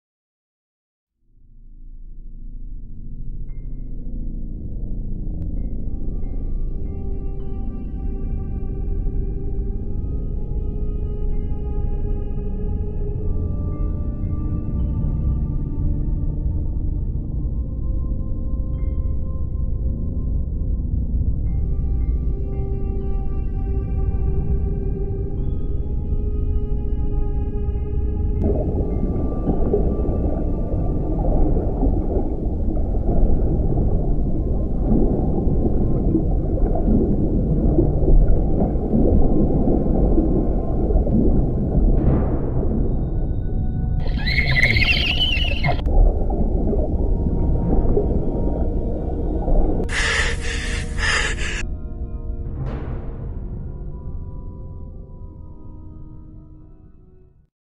Deep Forest Lake Location & Animation
Here's an eventual lake location for this scene that I've made the animated storyboard. But to find it around L.A. ! It's another question.